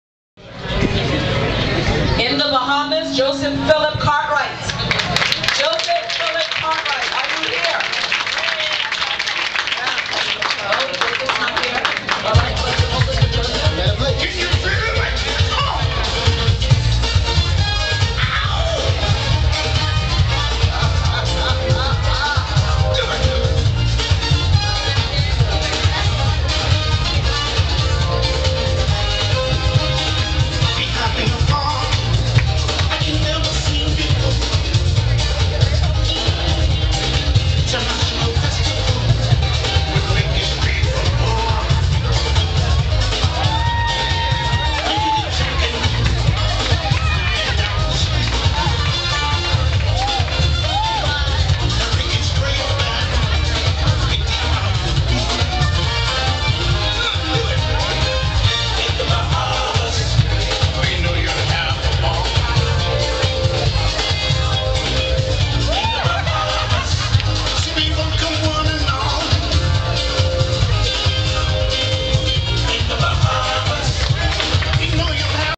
Da Bahamian Ting Song Competition Happy Hour - Song Pick #4
Song Pick #4 at Da Bahamian Ting Song Competition Happy Hour, 4:30 PM, Pompey Square, Nassau, Bahamas.